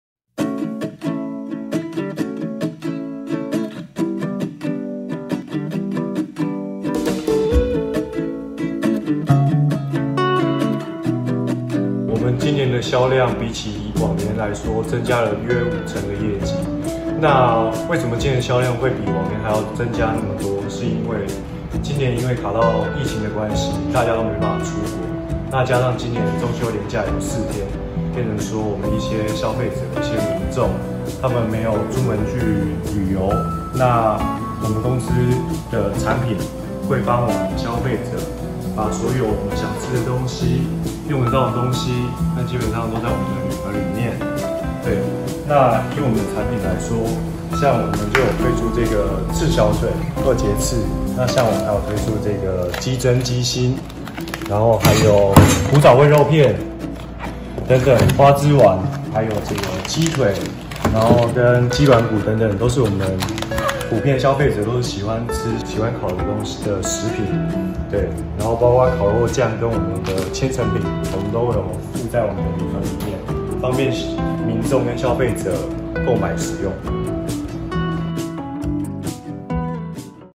新冠肺炎民眾在家團聚，𡘙師傅中秋烤肉禮盒意外熱銷｜20200928台灣民眾電子報
受到新冠肺炎疫情影響
國內知名連鎖便當業者𡘙師傅
往年在中秋節前夕推出的中秋烤肉禮盒今年業績意外爆紅
銷售量比以往增加五成。